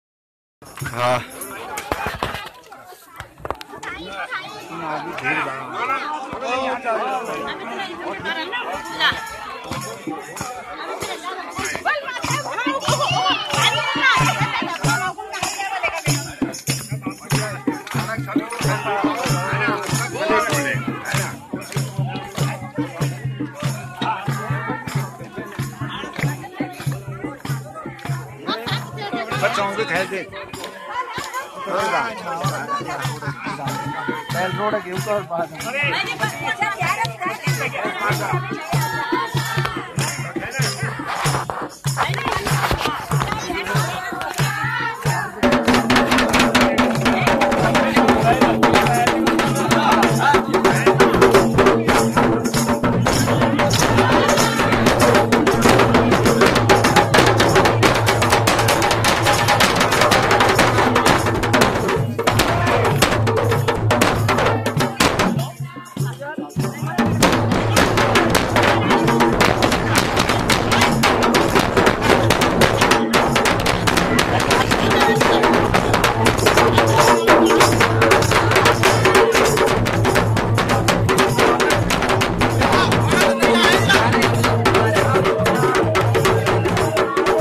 Gram Sabha Kheda main pehli Baar bus jaane ki Khushi mein kirtan bhajan dhol damau ek najara
Video from SuReSH RaWaT